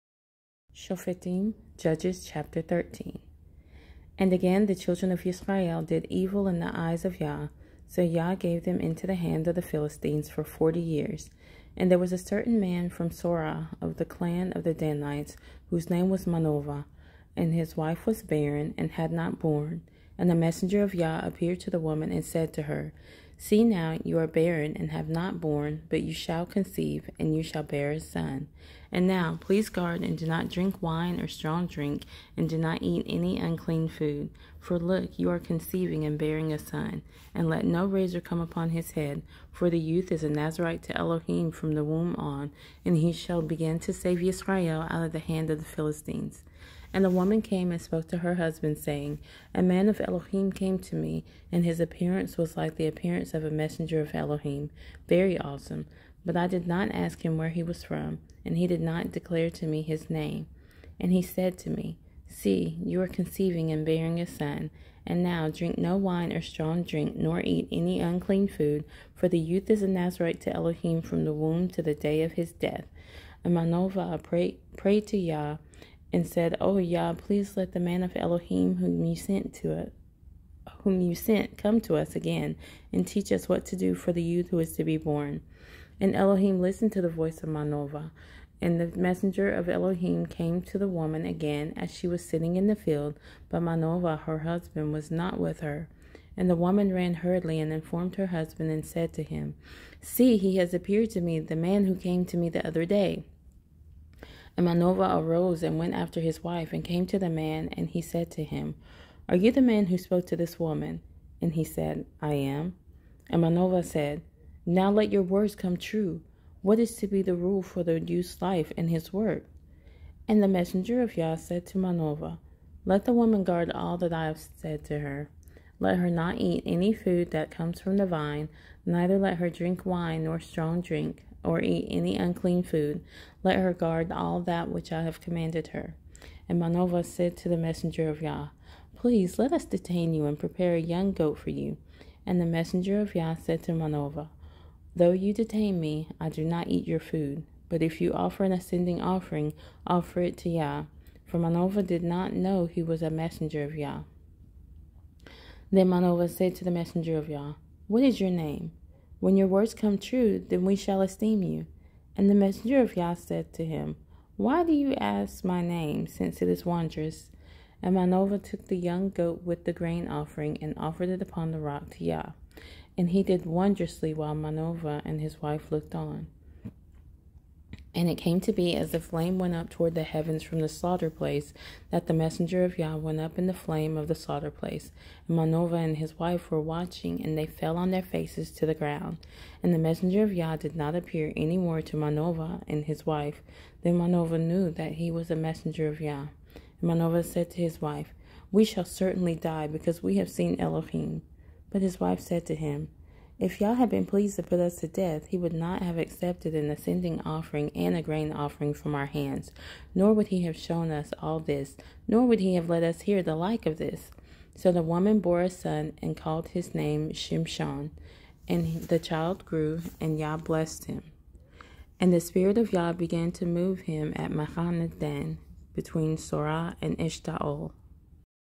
Shophetim/ Judges Chapter 13
Reading of the thirteenth chapter of Shophetim/ Judges from the You Version app TS2009 Version. New chapters/books will be uploaded daily.